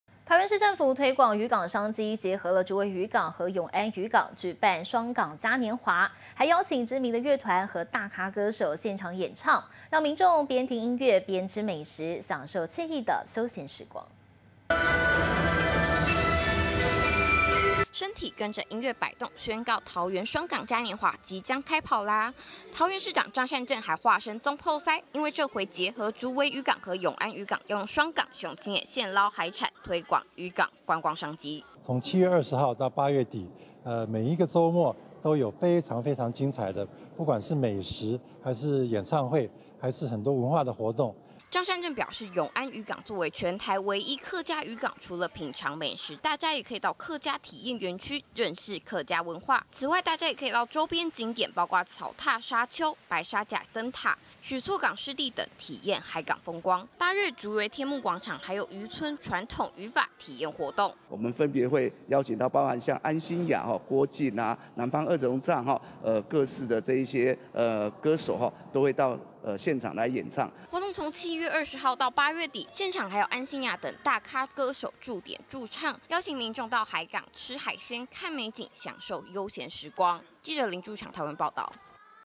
到漁港吹海風吃美食 張善政化身總舖師同樂
桃園市政府推廣漁港商機！結合竹圍漁港和永安漁港，舉辦「雙港嘉年華」，還邀請知名樂團和大咖歌手現場演唱，讓民眾邊聽音樂邊吃美食，享受愜意的休閒時光。